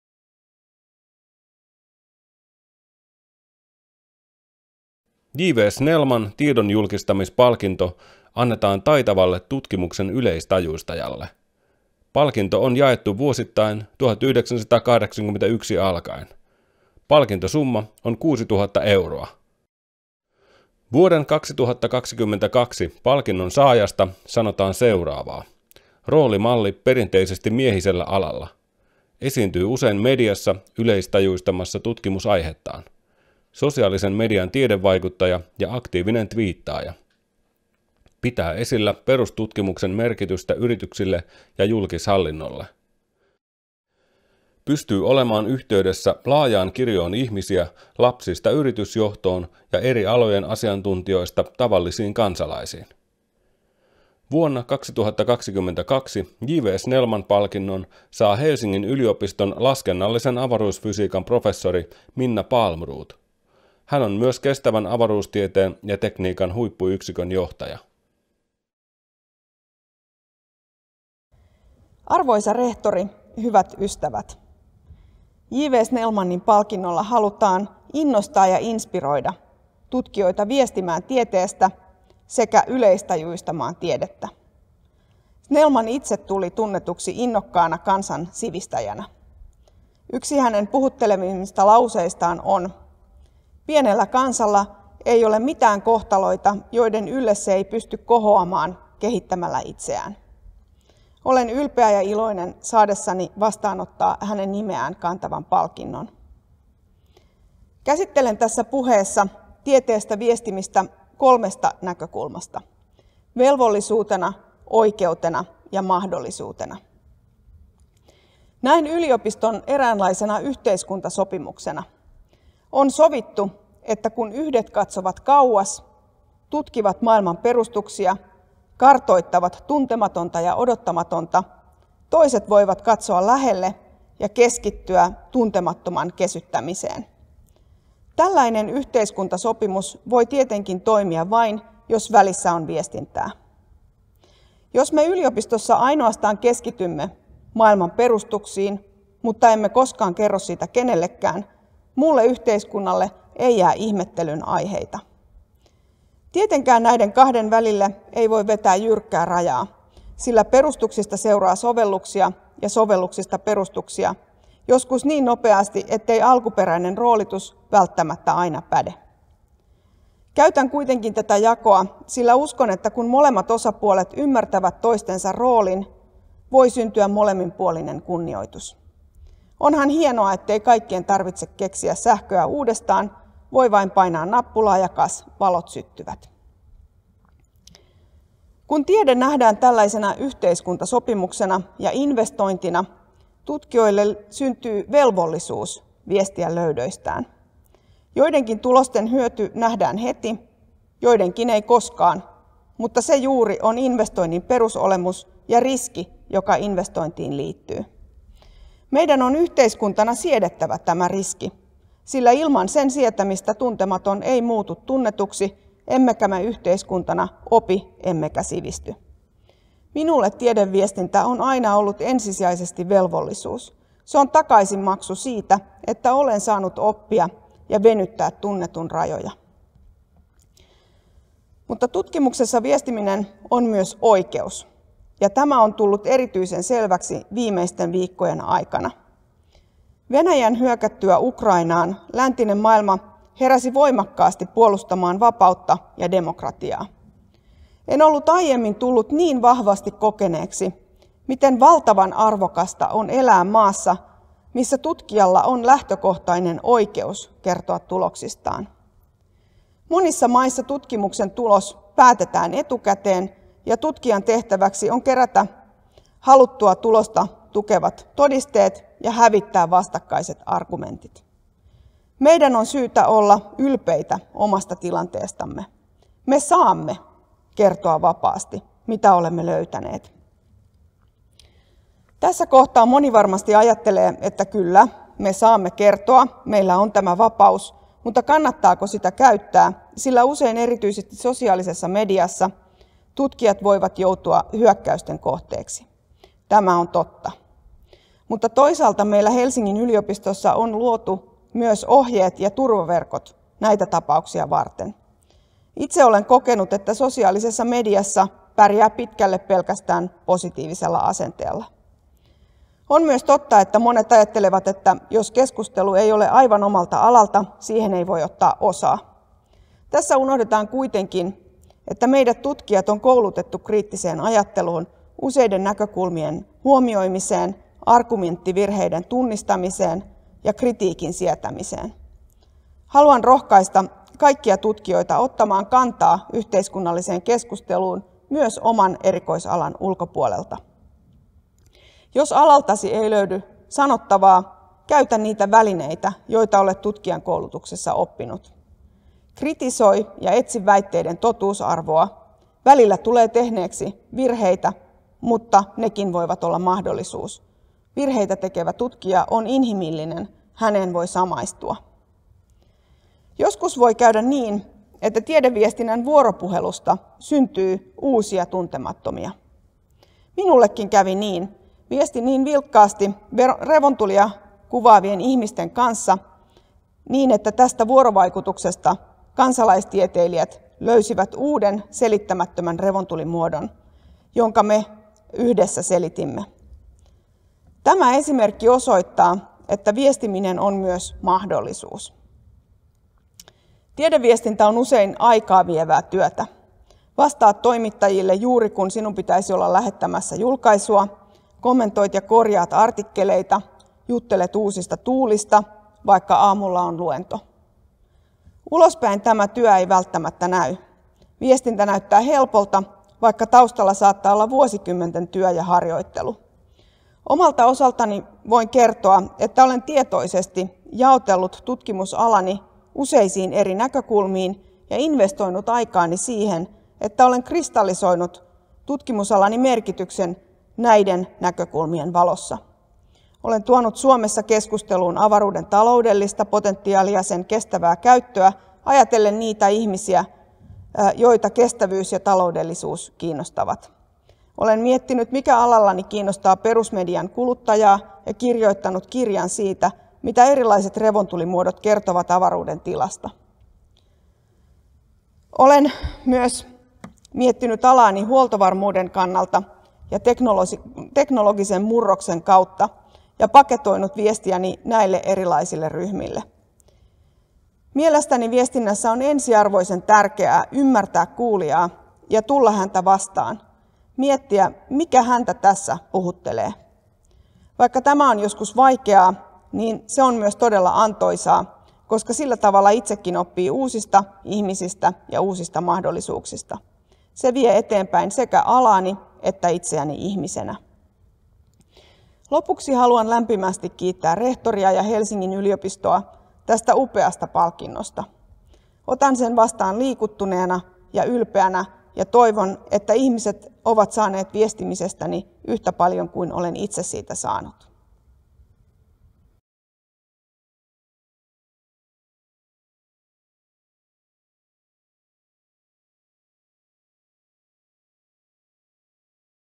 Helsingin yliopiston syntymäpäiväviikko: J. V. Snellman -palkinto 24.3.2022 | Helsingin yliopisto
Tieteen popularisoijalle jaettavan J. V. Snellman -palkinnon saajan julkistus ja palkitun puhe.
J.V. Snellman-prisets mottagare håller tal.
Speech by the recipient of the J. V. Snellman Award.